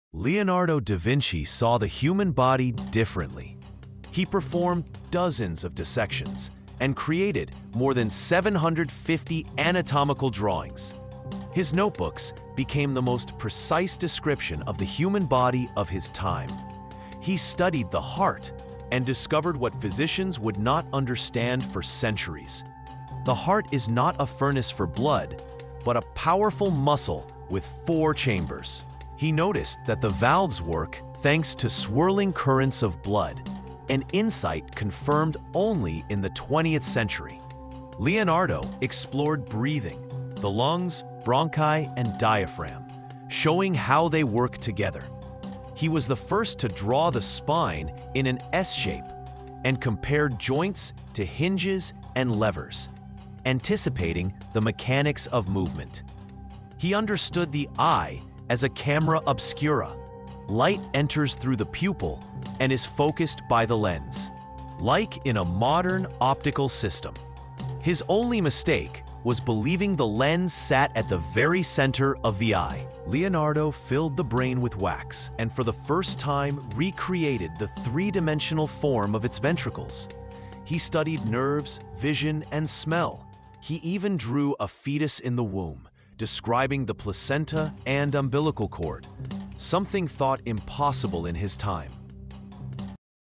Leonardo da Vinci: The Human Body, Revealed | Episode 4
Leonardo da Vinci saw the human body differently. In this episode we show how he dissected in secret, mapped the heart as a four-chamber muscle, sketched valve vortices (confirmed centuries later), treated the eye like a camera obscura, and modeled the brain’s ventricles in 3D.
Chapters:
0:00 Hook — why Leonardo’s anatomy still matters
0:12 Heart as a muscle, valve vortices
0:38 Lungs, diaphragm, and breathing mechanics
0:58 Eye as camera obscura (and his lens mistake)
1:18 Brain casts and early neuroanatomy